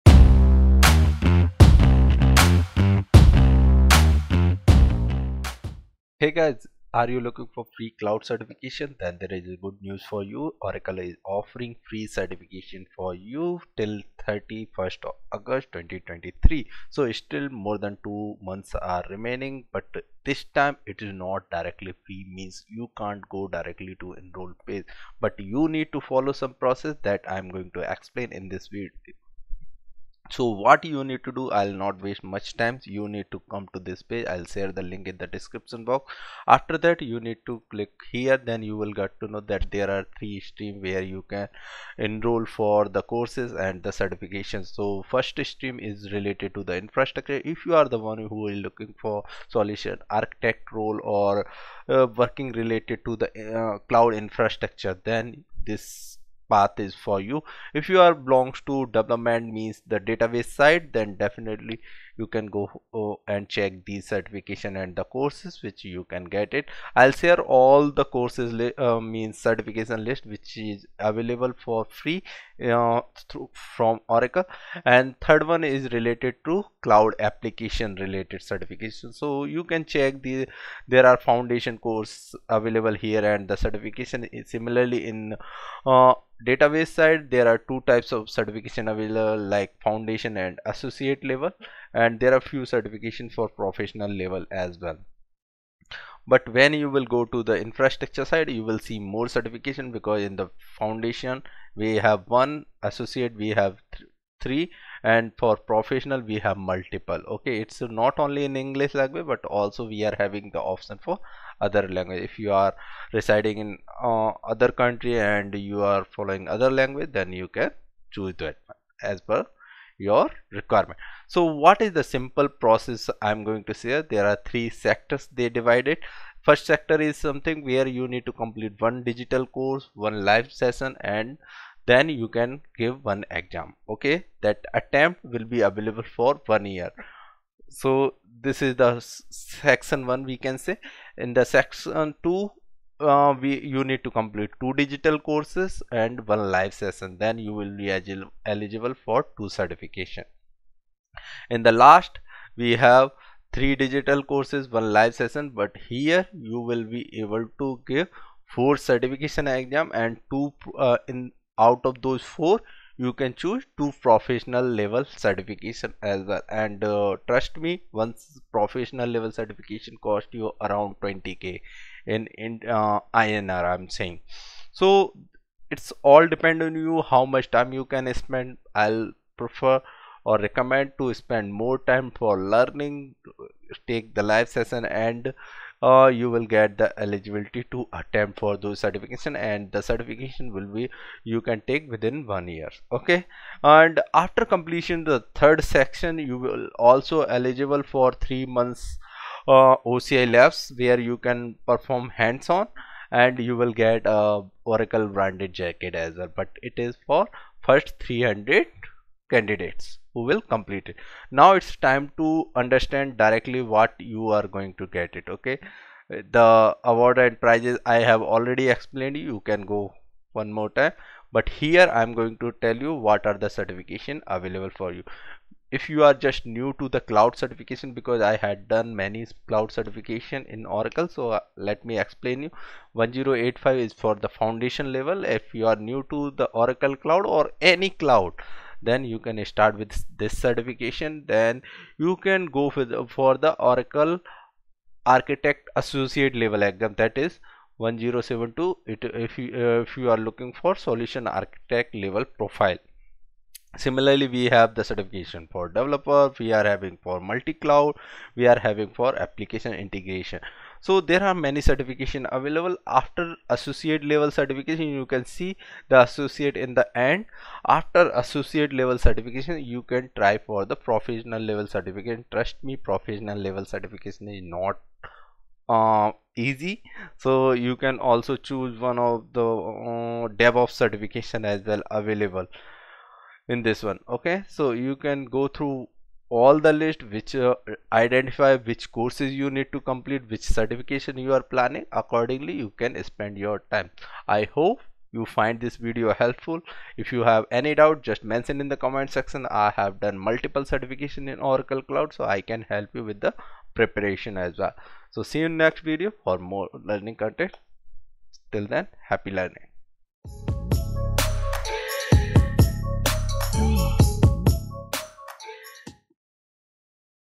Free Cloud Certification 2023 | Free OCI Certifications - Last Date 31st August 2023
In this video, I am going to talk about how to register for free cloud certification offered by Oracle for 2023. Great opportunity to upskill and grow in Cloud domain.
Foundation to Professional Certifications.